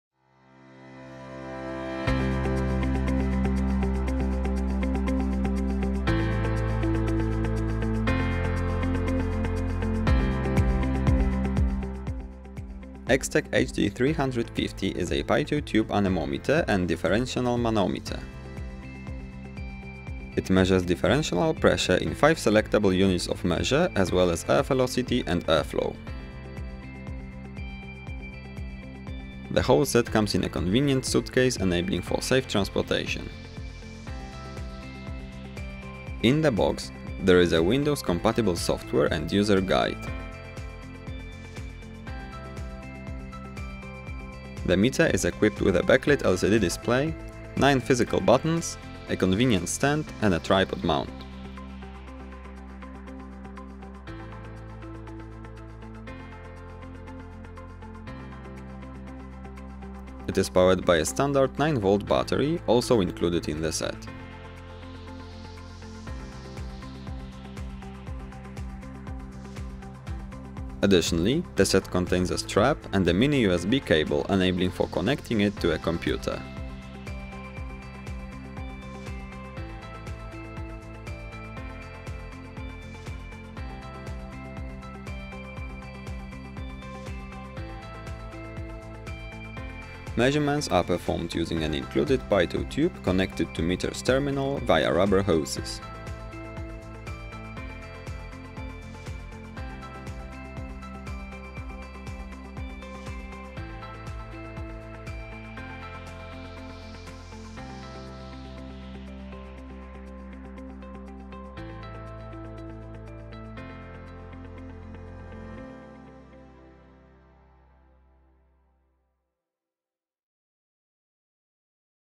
EXTECH HD350 - Thermoanemometer - UNBOXING
Type of meter: thermoanemometer
TME Symbol: HD350

Features:
• Velocity measuring accuracy: ±3%
• Temperature measuring range: 0...50°C
• Temperature measurement accuracy: ±1°C
• Temperature measurement resolution: 0.1°C
• Pressure measuring range: 0...50mbar
• Pressure measuring accuracy: 0,3% of full scale
• Measurement repeatability: ±0,2%

Velocity measuring range:
• 1...80m/s
• 2,25...178,66mph
• 2...154,6 knots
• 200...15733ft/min
• 3,5...288km/h

Velocity measuring resolution:
• 0,01m/s
• 0,01mph
• 0,1km/h
• 0,1knot
• 1ft/min

Additional information:
• Gross weight: 2873.9 g
• Guarantee (months): 24